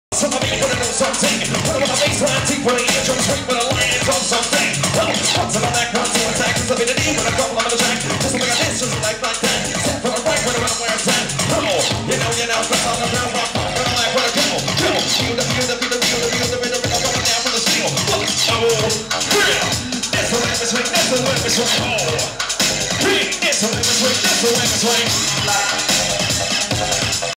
Liondub w/ MC Posi-D @ Revolution of Bass [Greenpoint, Brooklyn 9/24/10]
MC Posi-D alongside Liondub at Bass Fueled Mischief's Revolution of Bass in Greenpoint, Brooklyn.